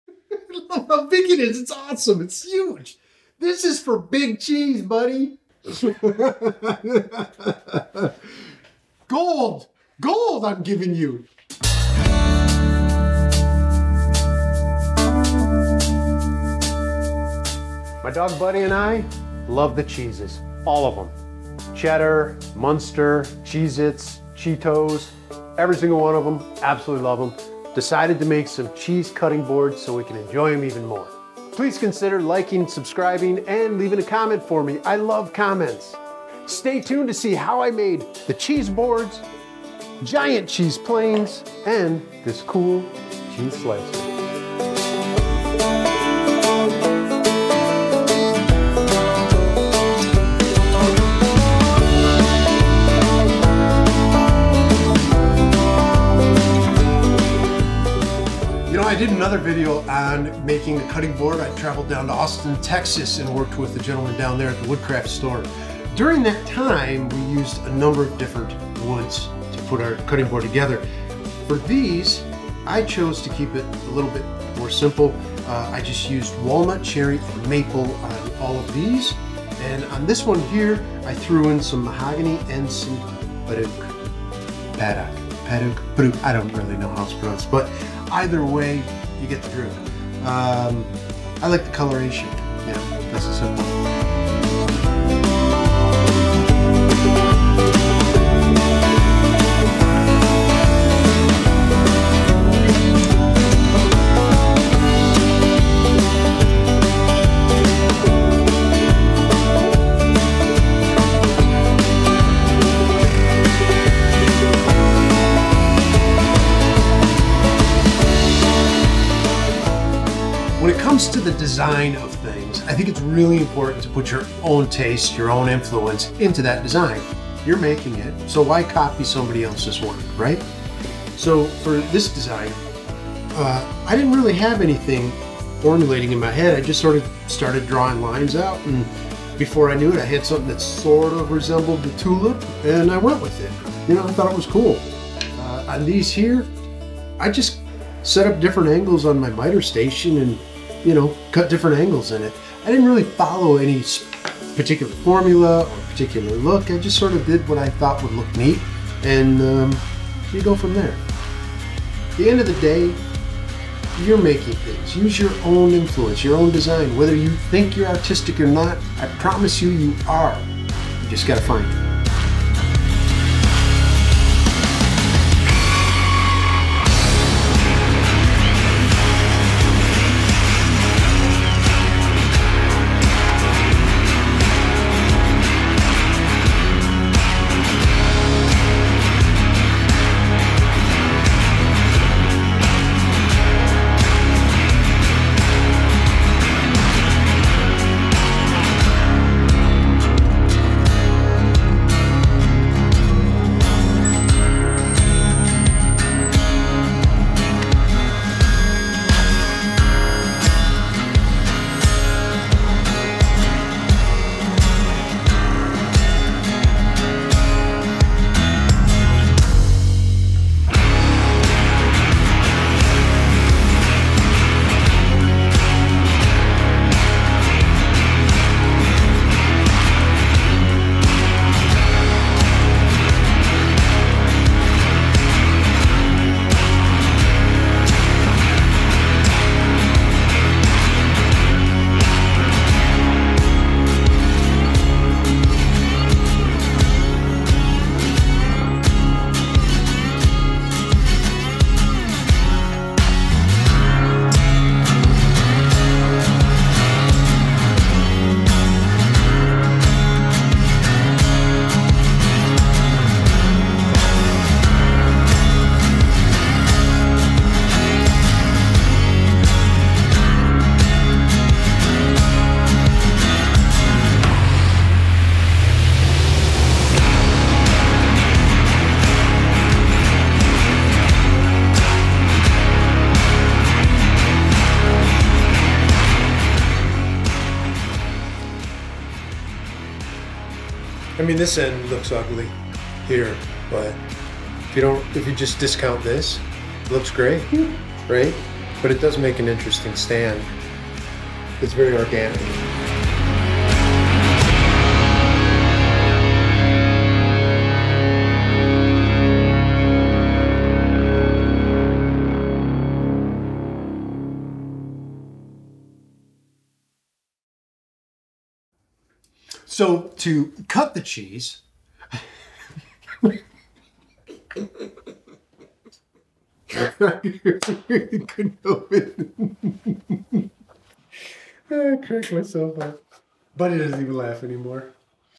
Making Cheese Boards, Cheese Planes and Cheese Slicers | Woodcraft 101
The ULTIMATE cheesy video.  It's time for you to learn how to handle Big Cheese.  Every party needs some cheese and I'm going to show you how to deliver the goods by making Cheese Boards, Cheese Planes and Cheese Slicers.

MATERIALS:
Walnut
Cherry
Maple
Mahogany
Padauk

KITS:
WoodRiver Cheese Plane Kit:

FINISHES:
General Finishes Butcher Block Oil